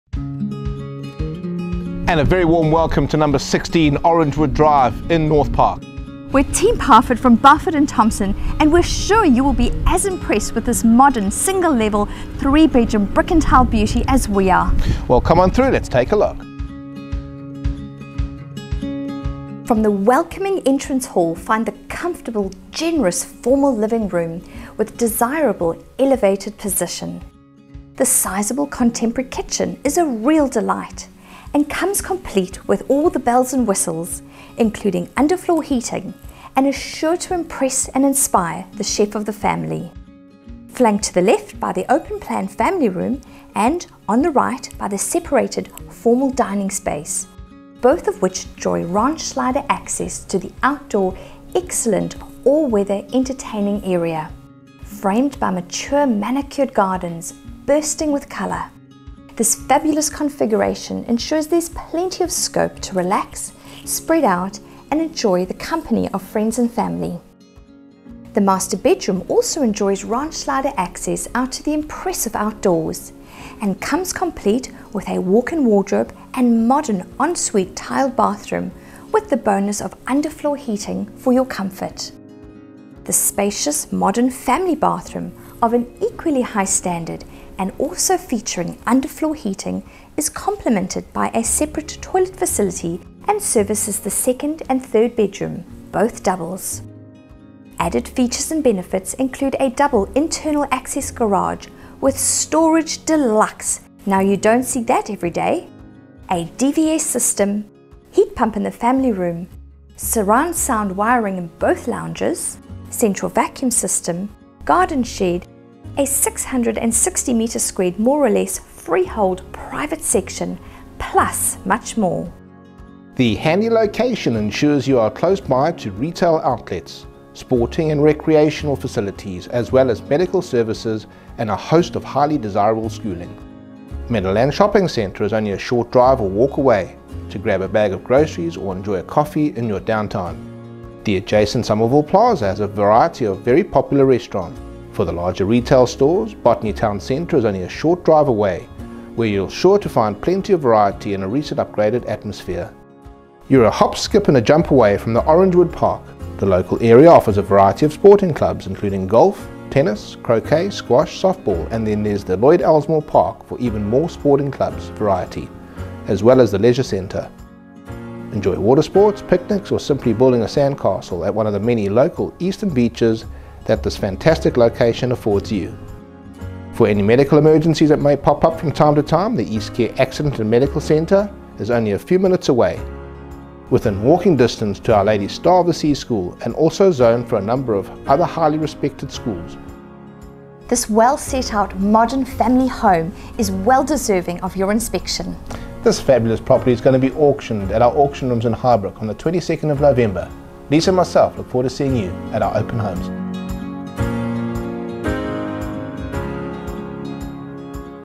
SOLD - 16 Orangewood Drive, Northpark - Lisa Parfitt and Wayne Parfitt
200 m2 Renovated Single Level Brick & Tile Beauty

This single level, brick & tile, family gem offers a spacious 200 m2 (mol) footprint, boasting a renovated interior, coupled with a convenient location. From the welcoming entrance hall, find the ...

Property Marketed by: Lisa Parfitt, Meadowlands and Wayne Parfitt, Meadowlands